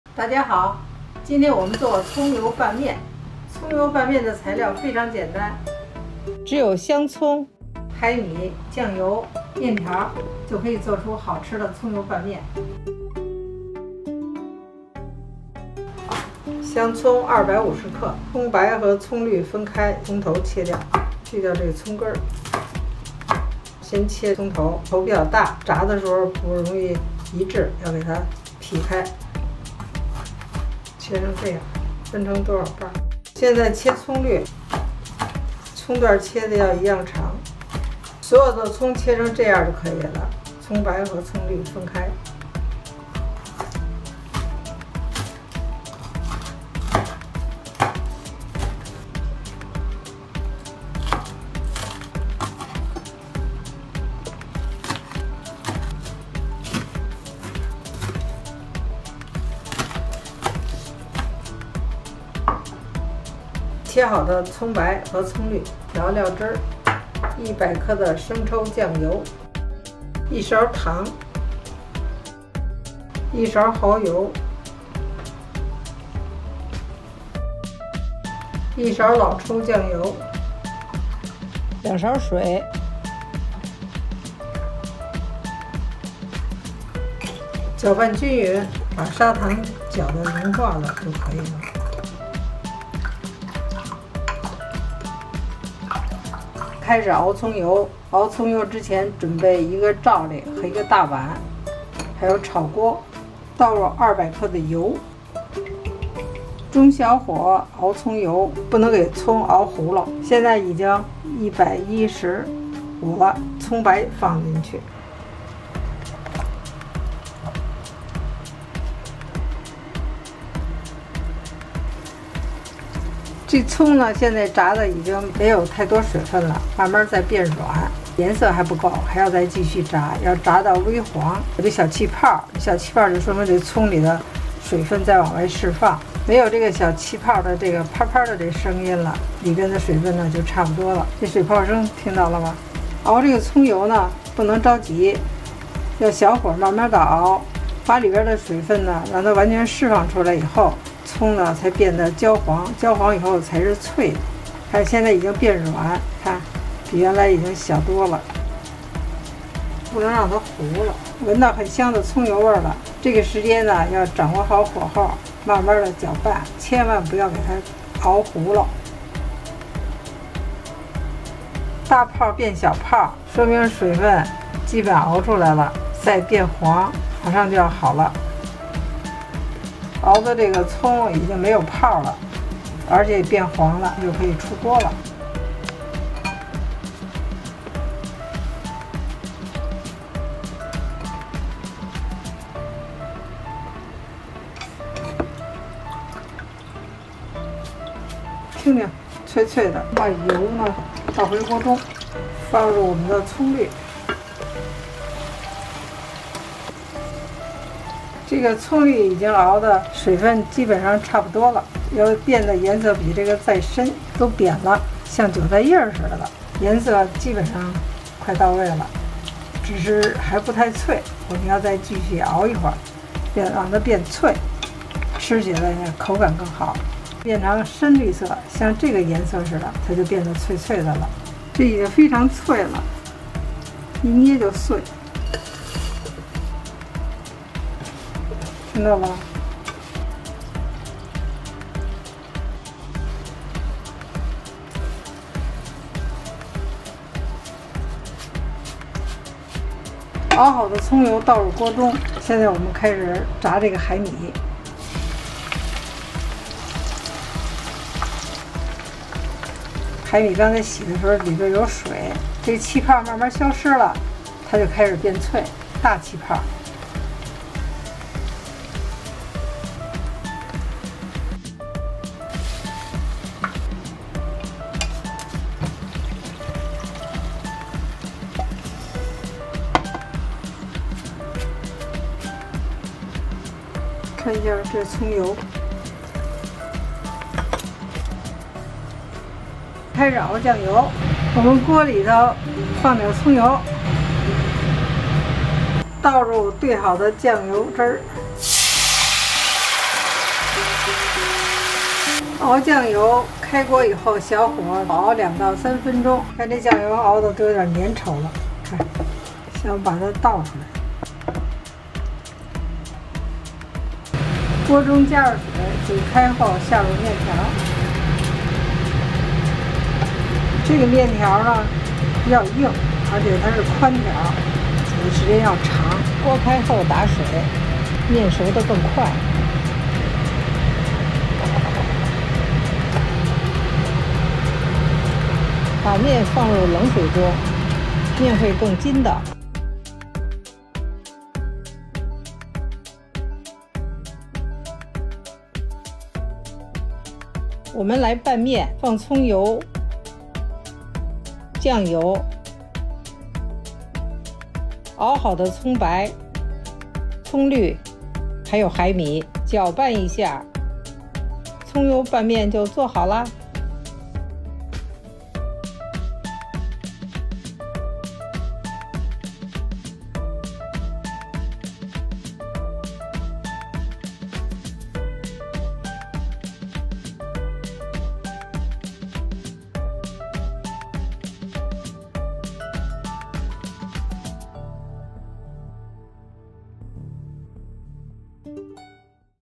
From the Streets of China: Authentic Scallion Oil Noodles Recipe | 来自中国的街头美食：正宗葱油拌面食谱
When you taste Mom's secret recipe scallion oil noodles, you will be deeply attracted by its rich aroma and irresistible taste, as if you are back in a street noodle shop in Shanghai. Although the dish is simple to make, requiring only boiling noodles and mixing scallion oil sauce, every detail is crucial, and only Mom's experience can bring the perfect taste. In this video, she will demonstrate the steps to make scallion oil noodles in detail, so you can easily make this classic and authentic dish at home. Follow Mom's video and let's make this mouth-watering scallion oil noodles together!

❤️ Scallion Oil Noodles
🥘 Ingredients
- 200g scallions
- 100g dried shrimp
- 200g oil
- 5 tbsp soy sauce
- 1 tbsp dark soy sauce
- 1 tbsp sugar
- 1 tbsp oyster sauce
- 2 tbsp water
- 1/2 pack (250g) dried wheat noodles

当你品尝妈妈的秘制葱油拌面时，你会被那浓郁的香气和诱人的味道深深地吸引住，仿佛回到上海的街头面店。这道美食虽然制作简单，只需要煮面和调制葱油酱，但是每一道细节都是关键，只有妈妈的经验才能带来完美的味道。在这个视频里，她将为您详细演示制作葱油拌面的步骤，让您在家也能轻松制作出这道经典又正宗的美食。快来跟随妈妈的视频，一起制作这道让人垂涎欲滴的葱油拌面吧！

❤️ 葱油拌面
🥘 材料
香葱250克、干虾米、油200克、酱油100克、糖1勺、老抽1勺、蚝油1勺、清水2勺、挂面半250克